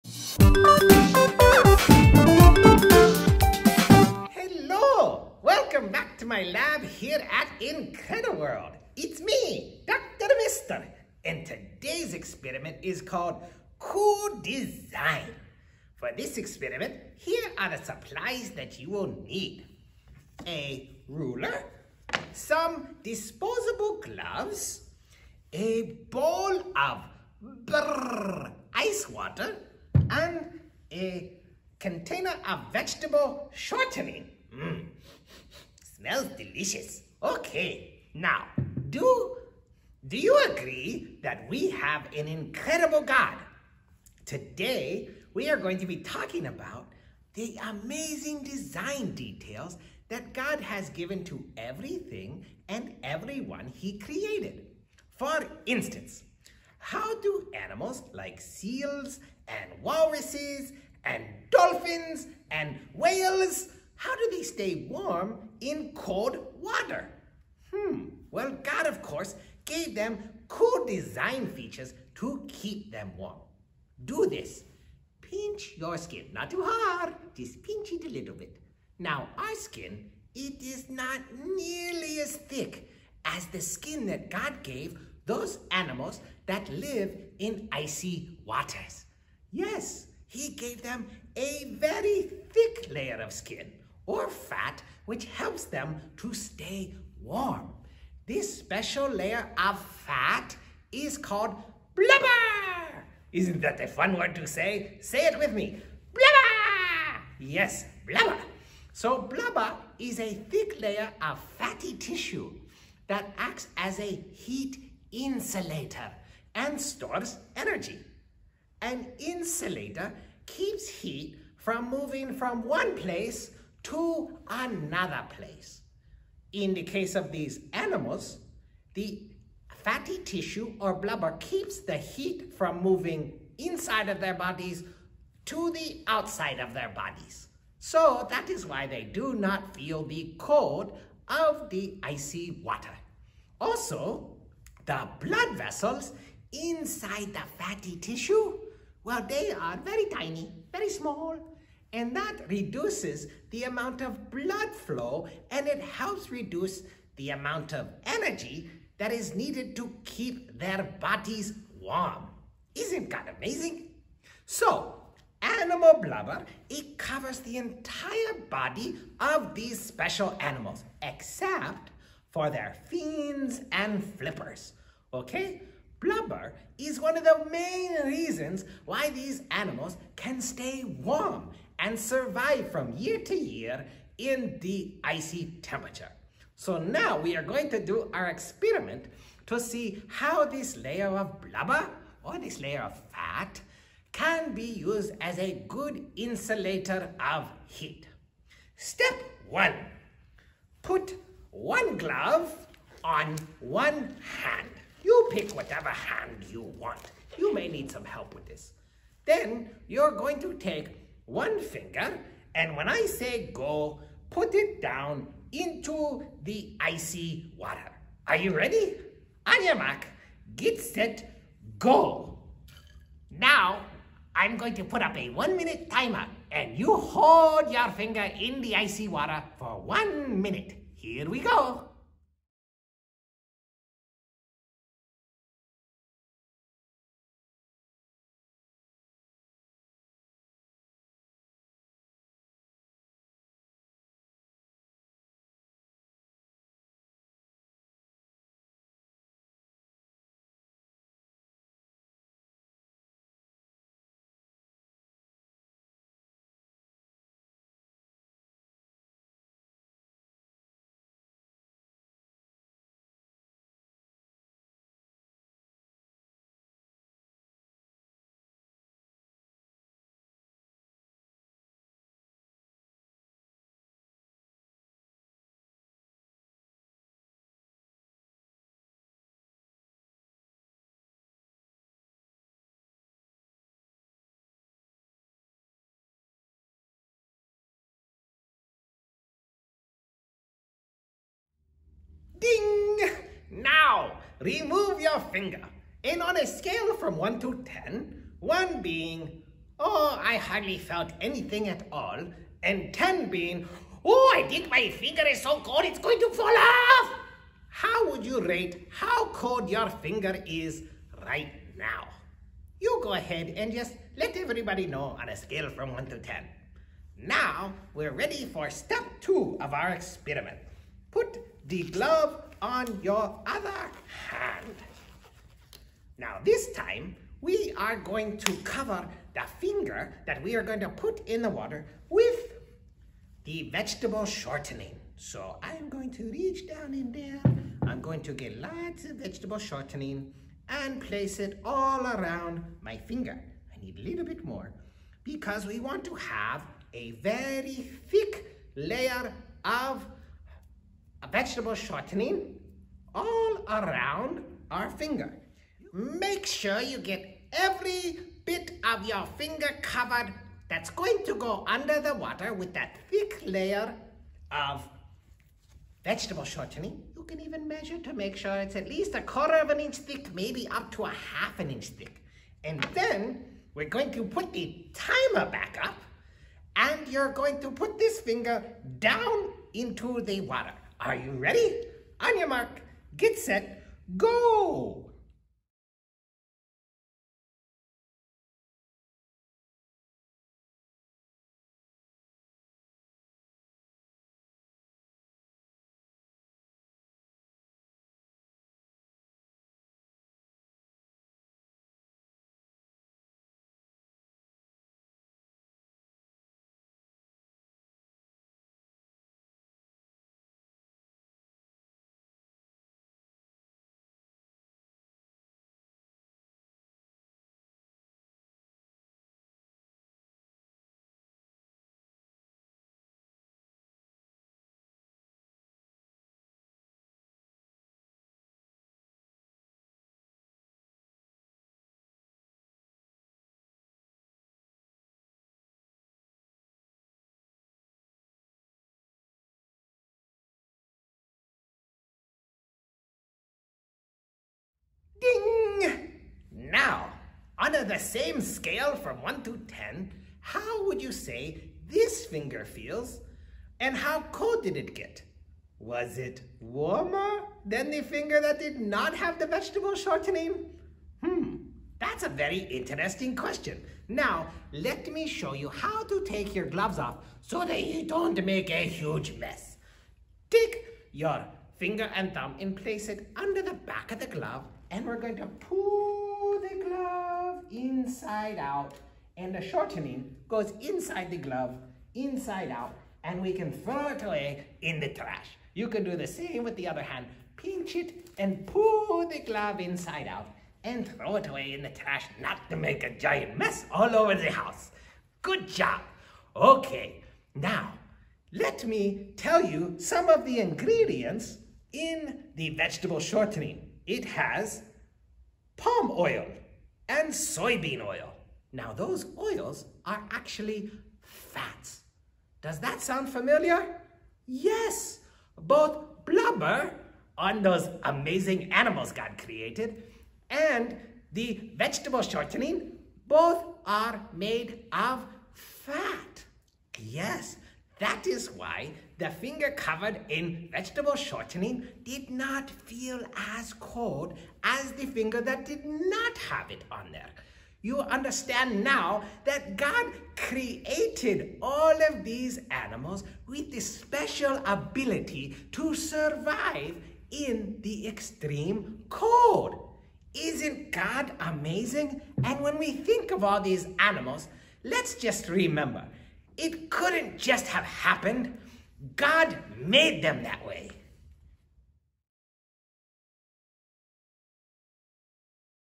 IncrediWorld Science Experiment Day 4
This is the Cool Design science experiment for Day 4 at IncrediWorld Amazement Park, an Answers VBS program.